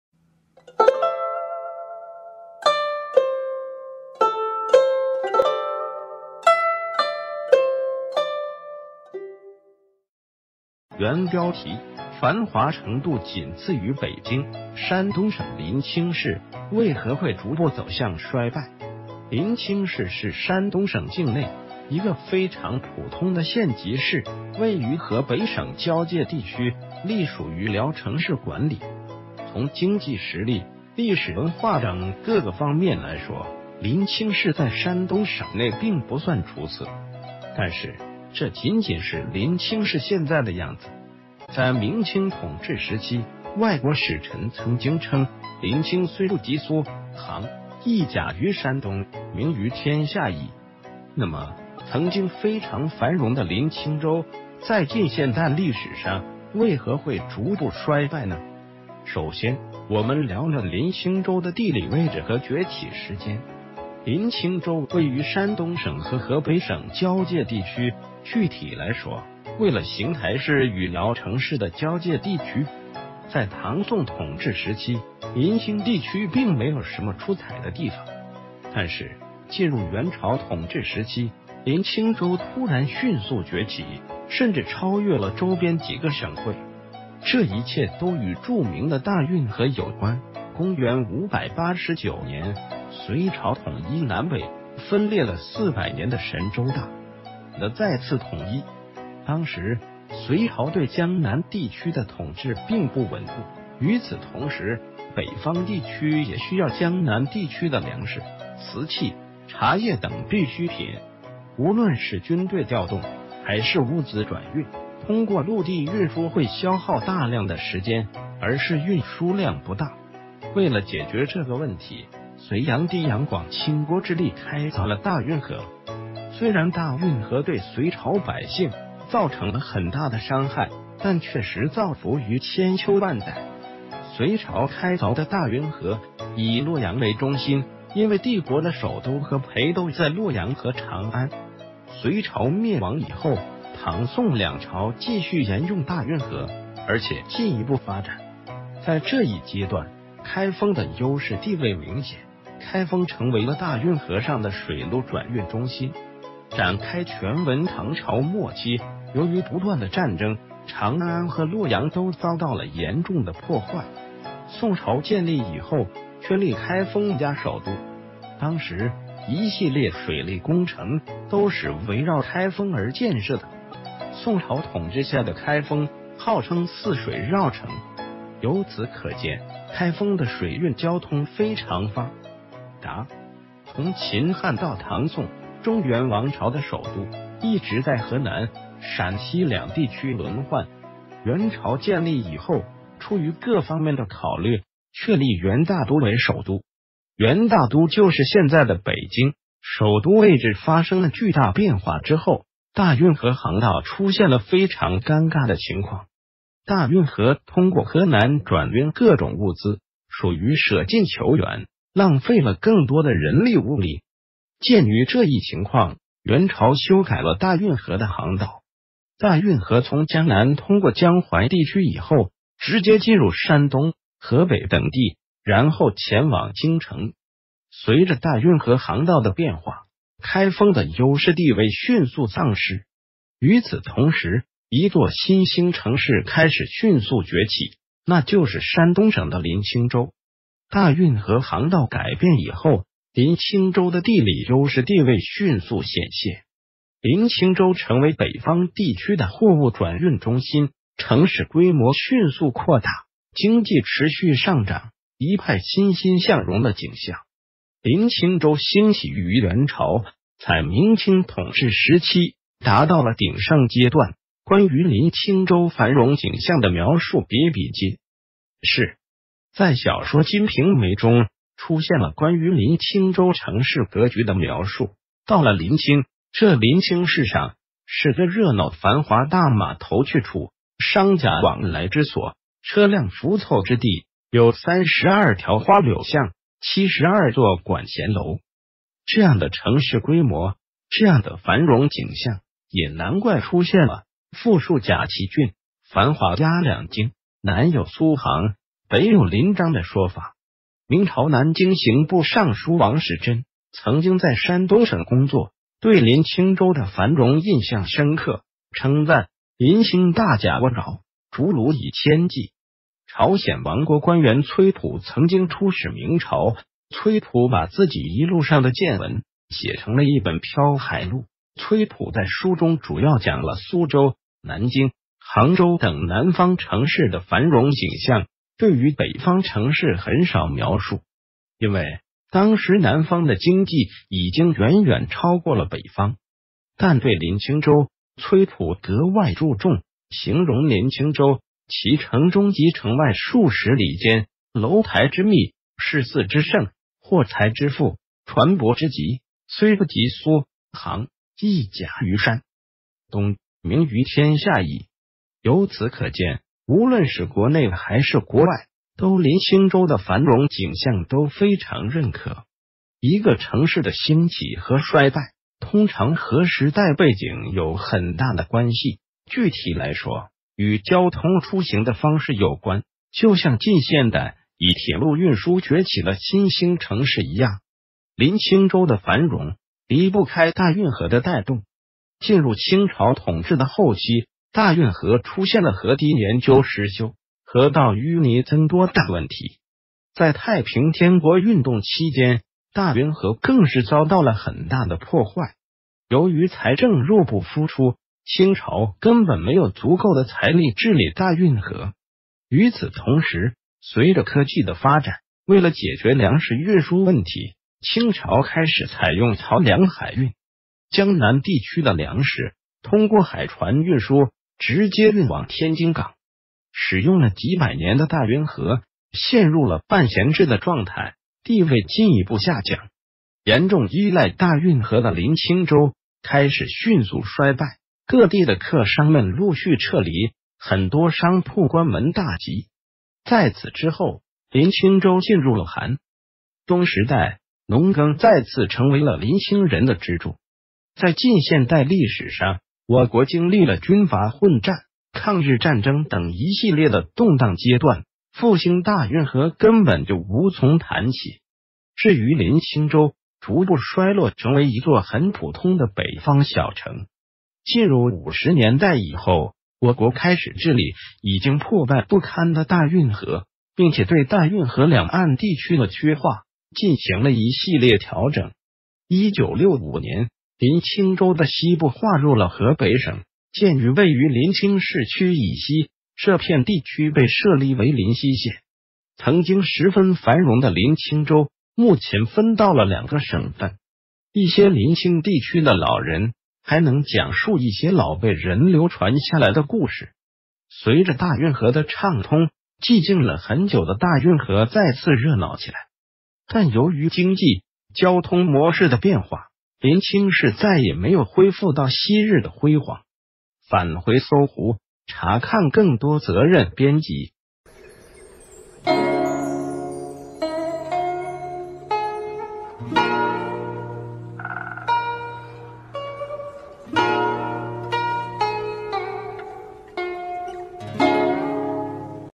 繁華程度僅次于北京，山東省臨清市，為何會逐步走向衰敗？_大運河
原標題：繁華程度僅次...